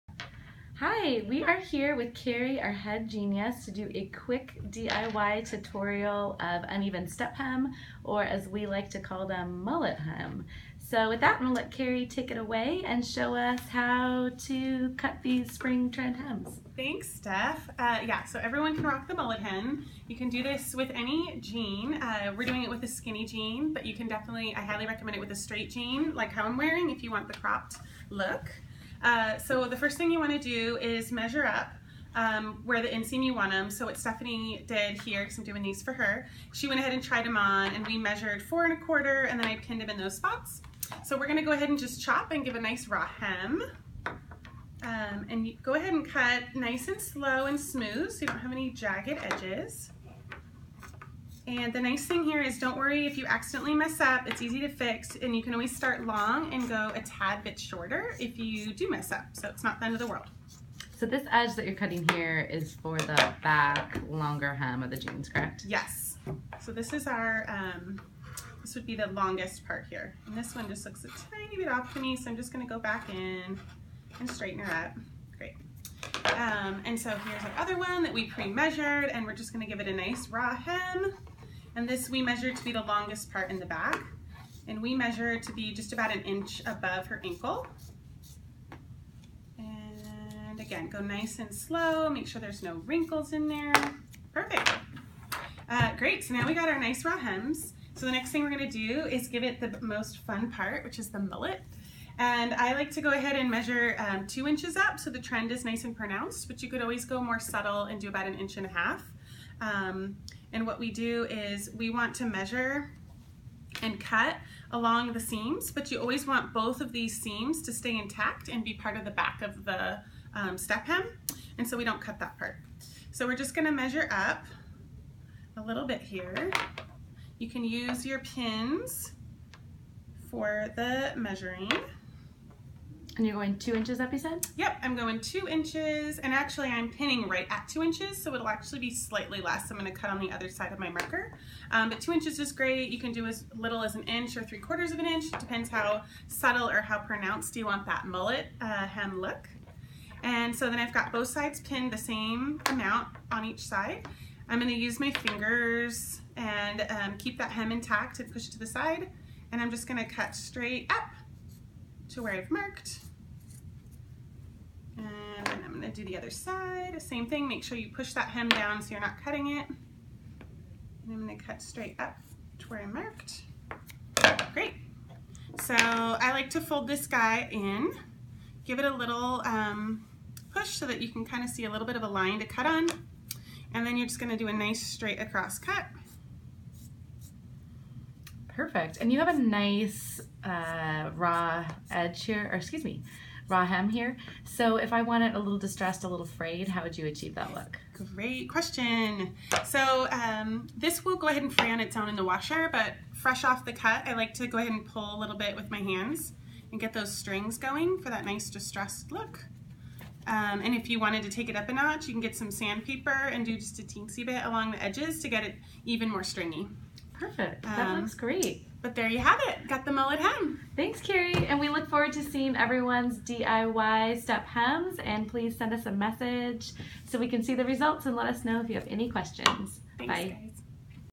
Uneven step hem (mullet hem) denim DIY
Want to know how to DIY the mullet hem that you're seeing everywhere this spring? We got you.

Our head Jeanius, Cari, will walk you through a quick DIY tutorial on how to create your own uneven step hem jeans.

All you need is a pair of jeans (boyfriend or straight styles work best), sewing scissors, 4 pins or clamps, and a ruler.